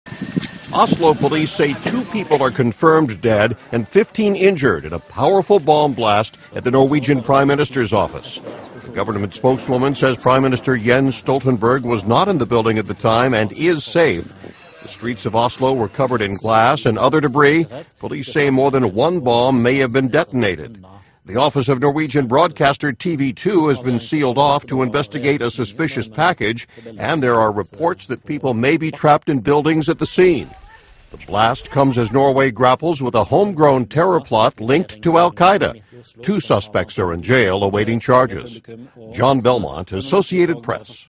Bomb Tears Through Government HQ in Oslo; 2 Dead
A powerful bomb tore into the heart of Norway, killing at least two people and injuring 15 as it ripped open buildings including the prime minister's office. (July 22)